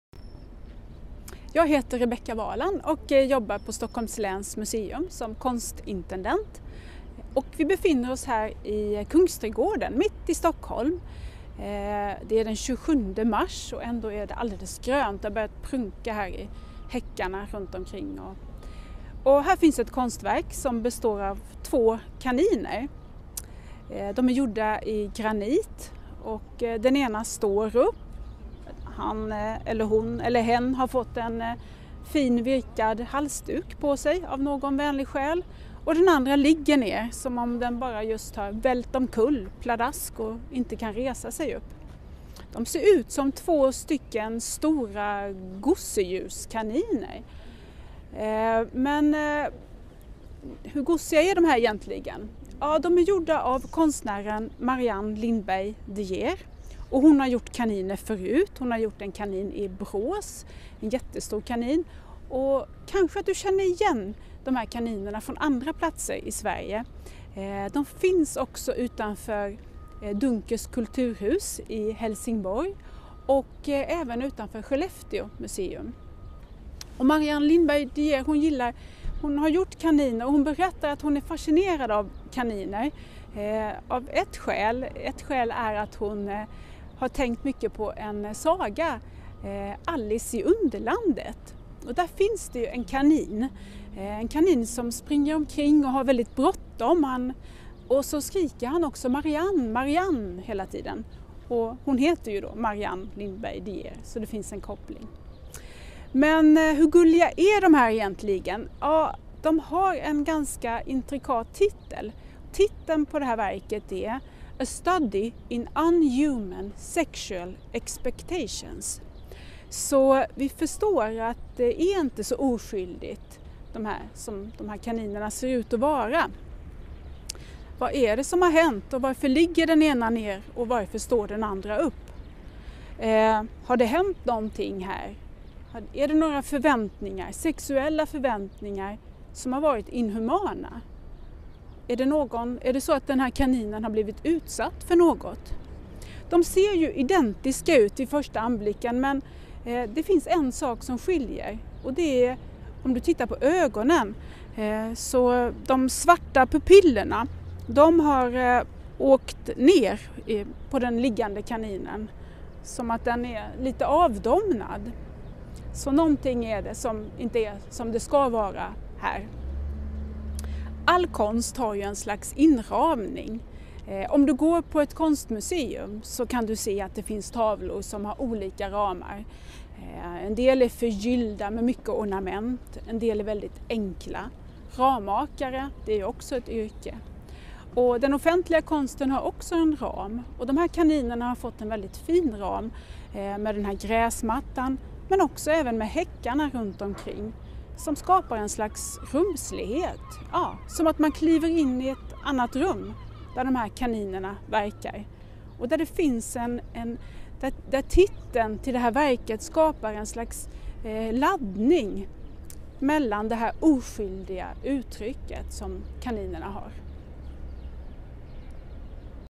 Upptäck länet– A Study in Unhuman Sexual Expectations
Konstintendent Rebecka Walan visar konstverket A Study in Unhuman Sexual Expectations av Marianne Lindberg De Geer. Konstverket ligger i Kungsträdgården, Stockholms stad.